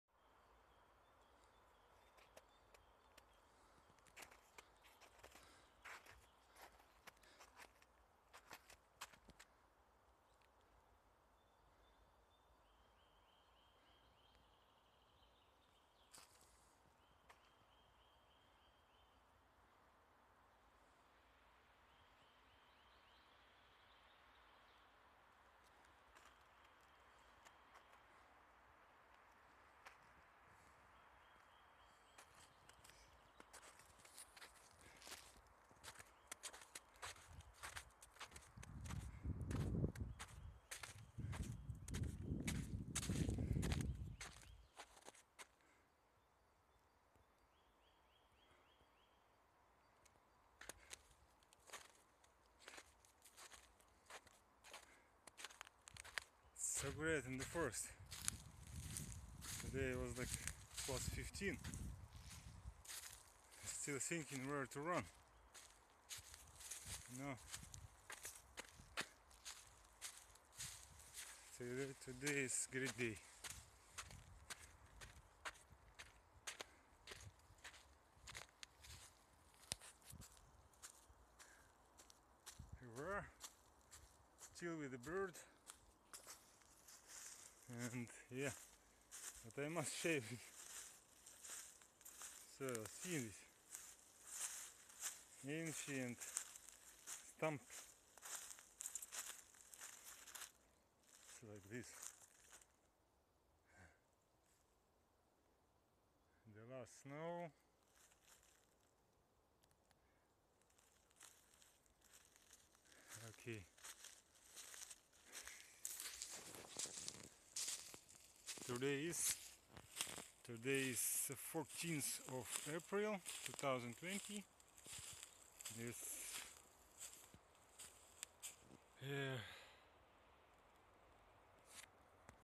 In the forest, melting more, moss-covered spring stumps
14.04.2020 / Near Akademgorodok of Novosibirsk, Russia.